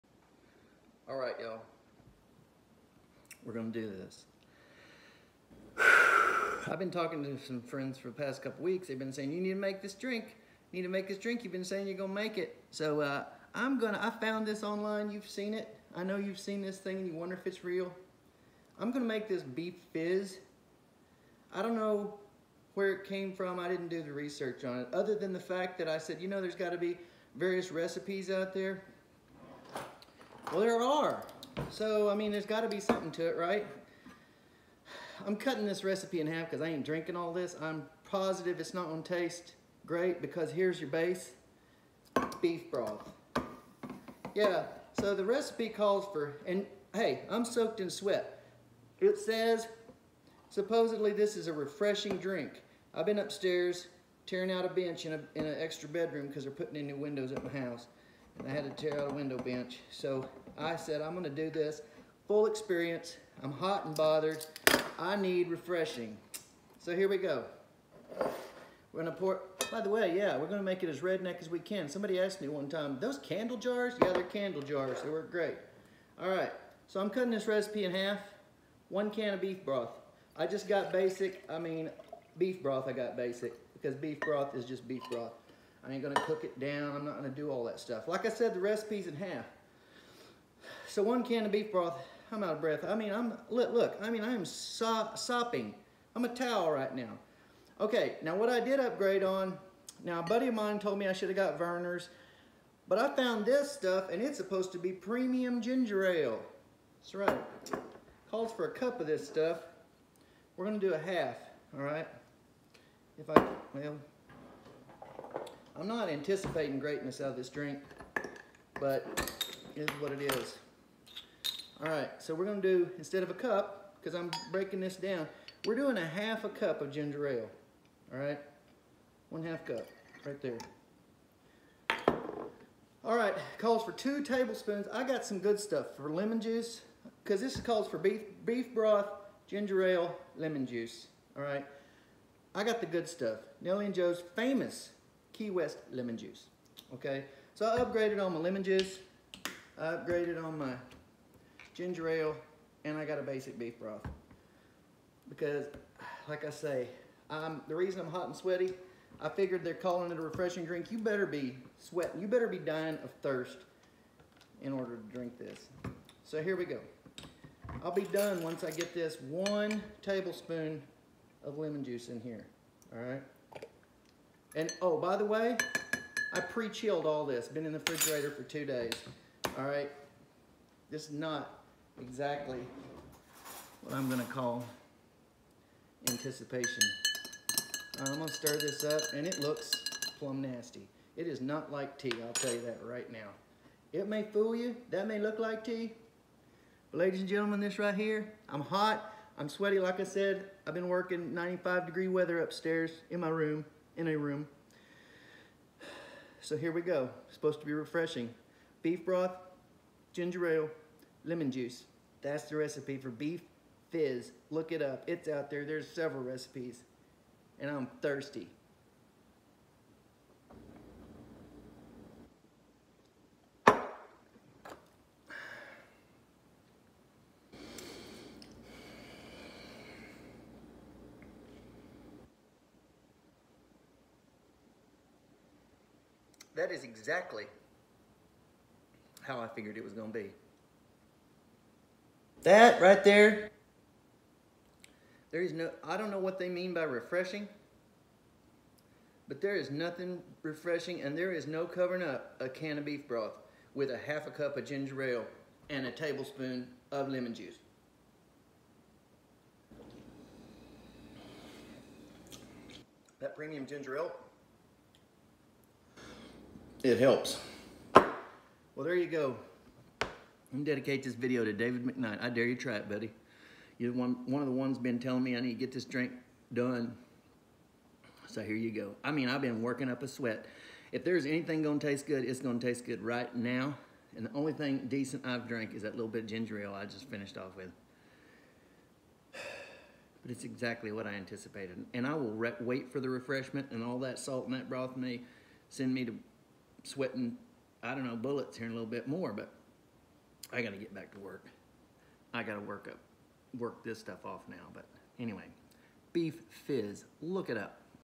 BEEF FIZZ?!?! “Refreshing,” they said...
Beef Fizz ... you have seen this recipe pop up on social media.  I thought it was just a meme, but apparently, judging by the various recipes I have seen...it has been around a long time.  I had to try it.  Make it for your friends...tell them you are going into the iced tea business.  ;)